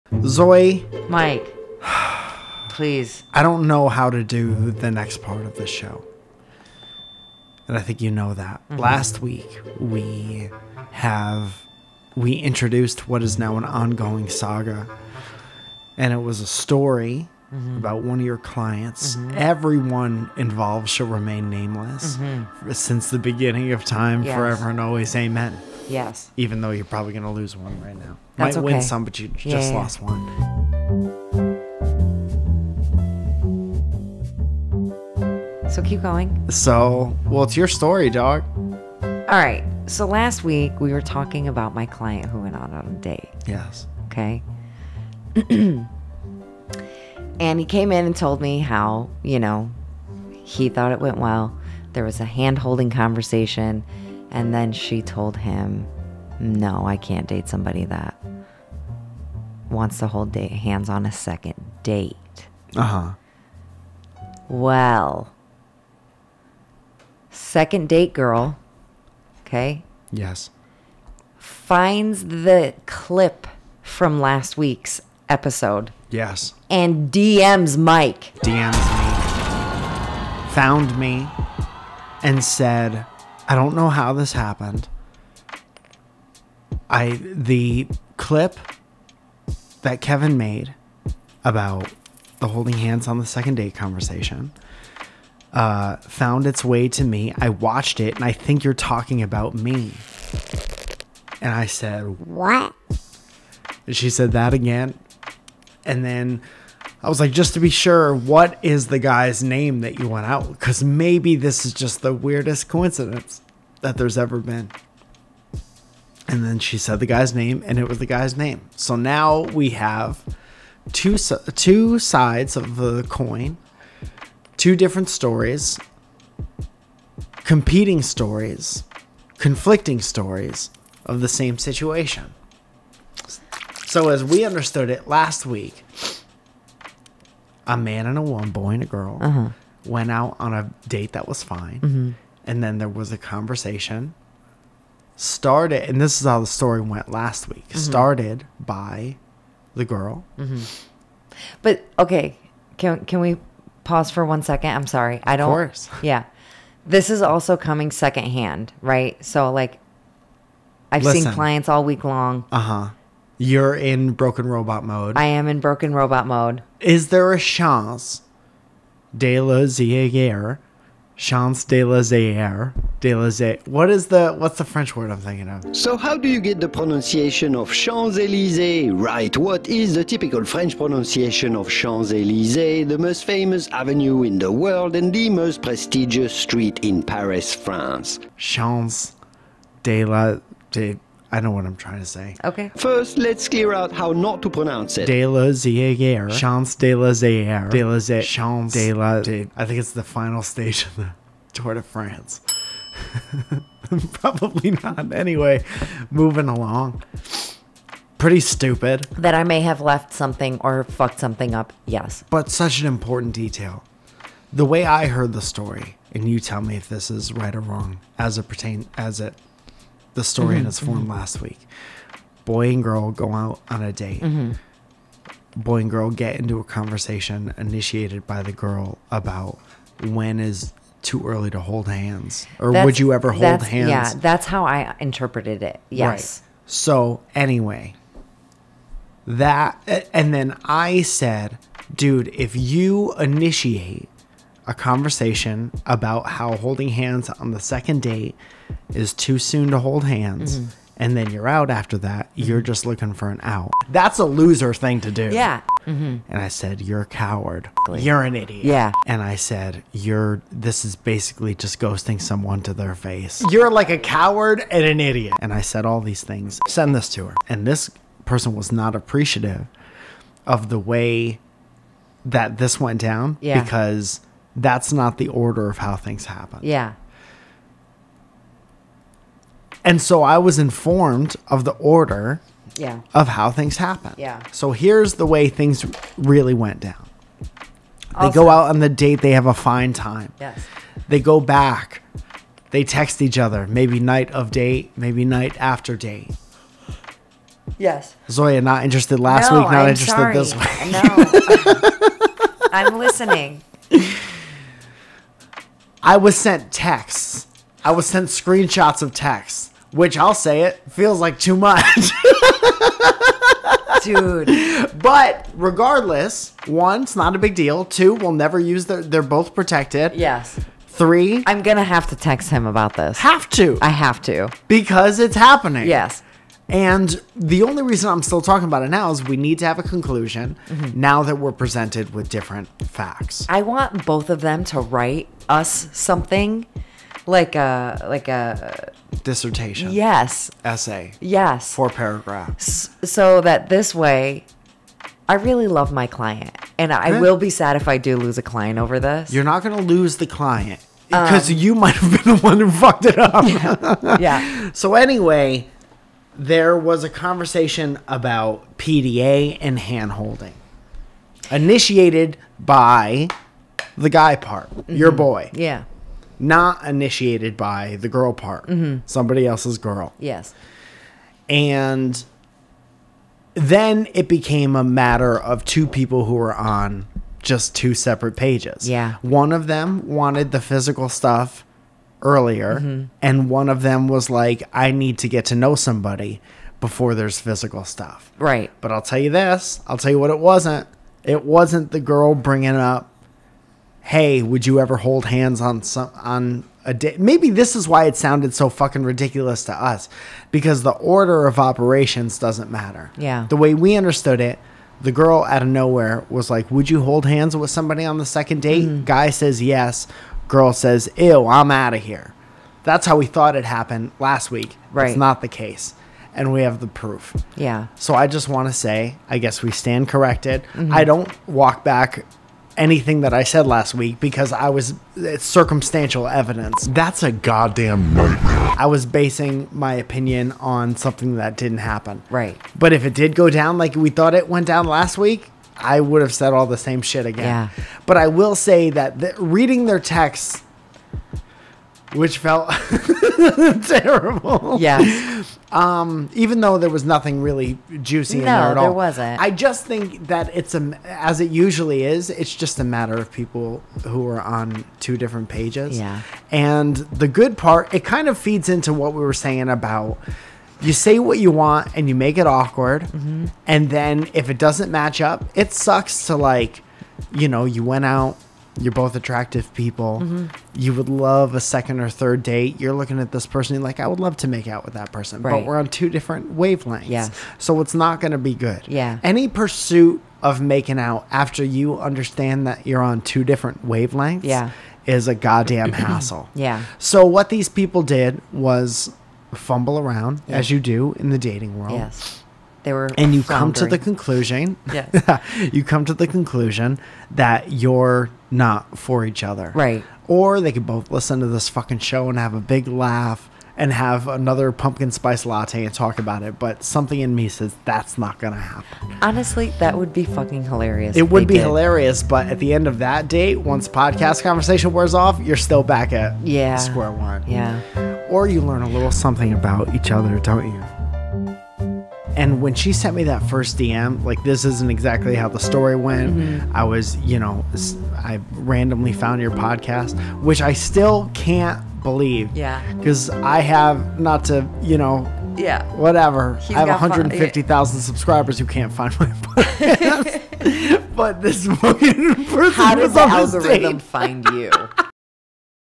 The Dangers of Second-Hand Storytelling | Welcome to Our Podcast Clip
Mike gets a DM from the girl that won't hold hands on a second date and the plot thickens! Edited by Kevin Plachy.

Everything we use for video production and podcasting

Josh Wilkinson: Just a sponsor that I like to mess with